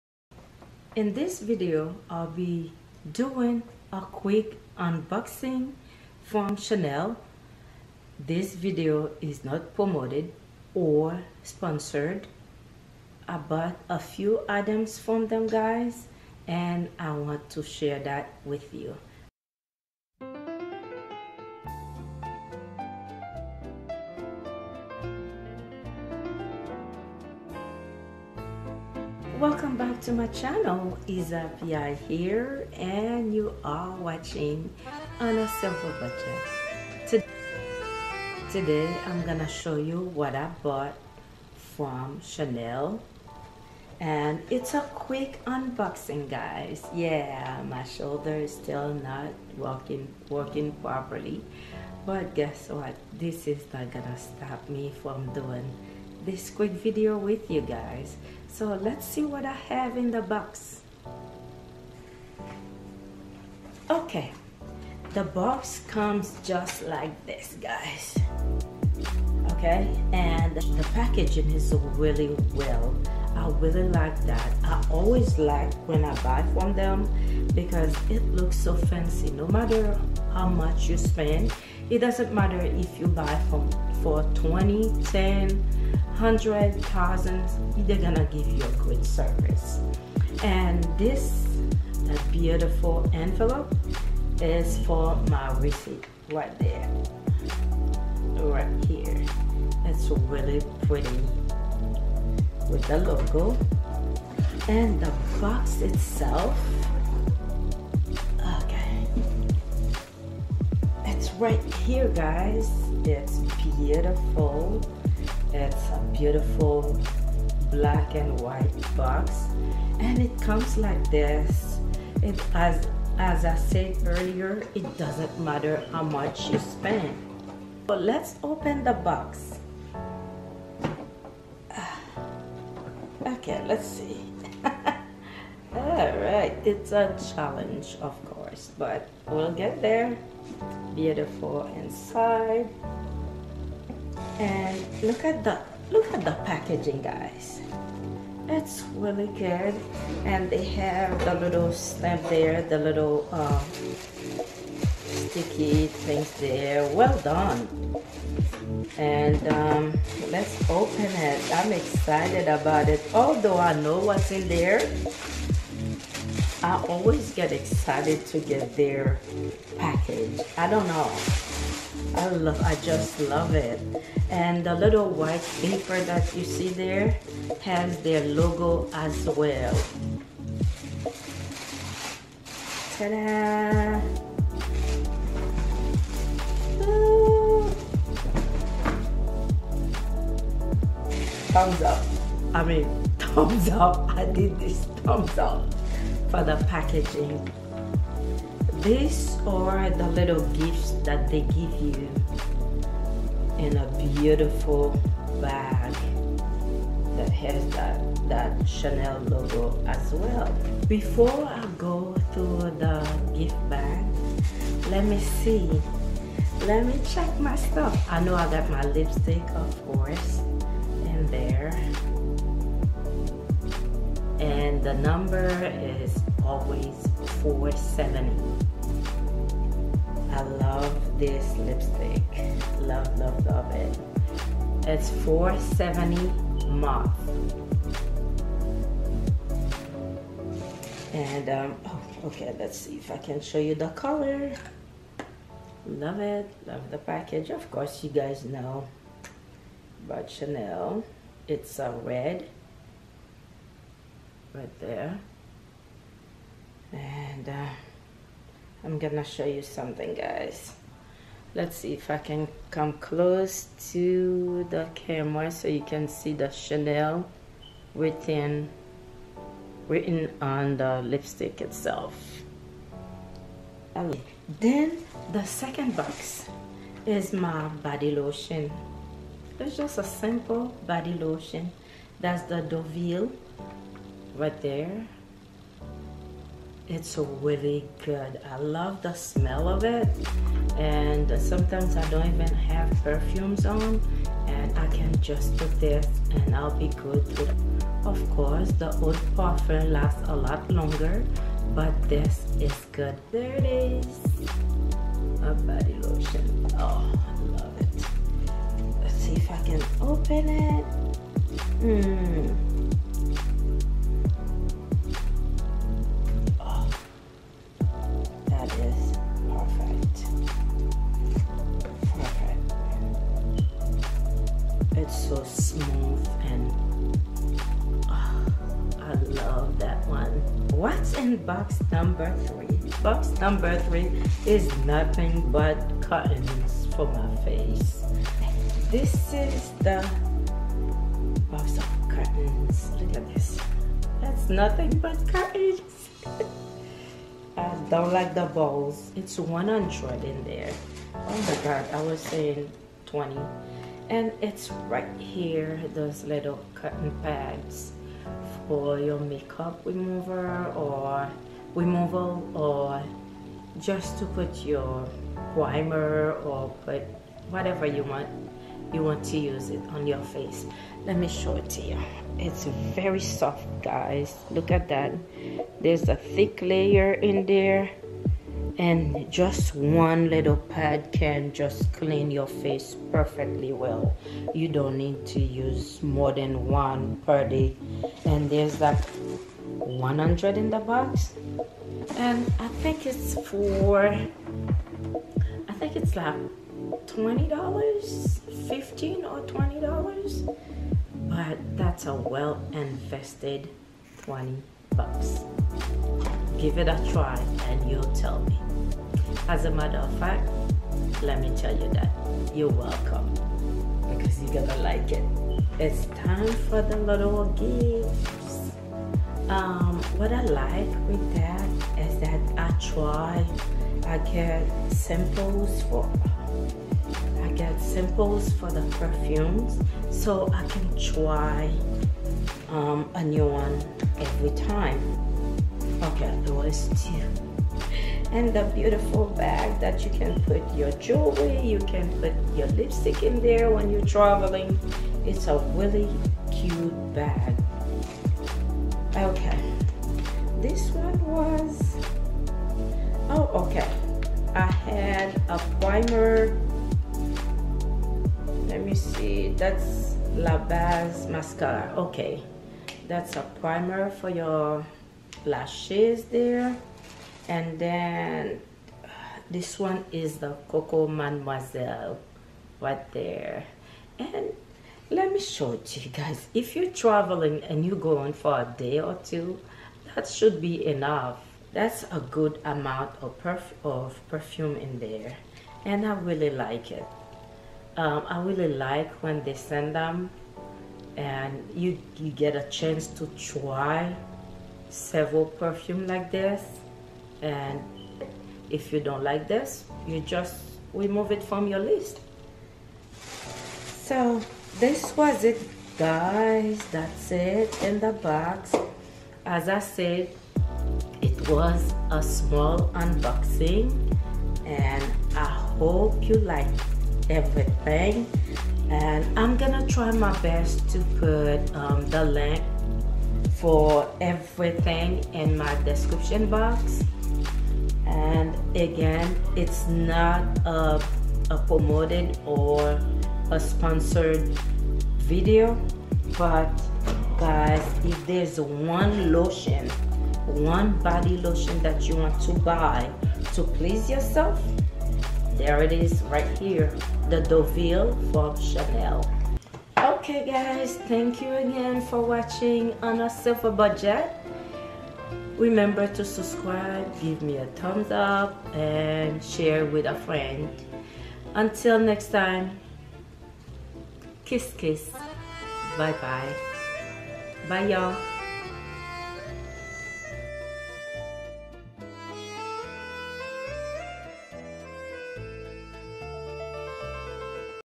Chanel unboxing, unboxing CHANEL, Deauville body lotion, Luxurious lotion
Hi guys, i bought a few items from CHANEL on line and, wanted to share my experience with you. It's not much but they are some of my favorites. I hope you enjoy and try them.

My address:
P.O.BOX 24144

Instagram: @On a Silver Budget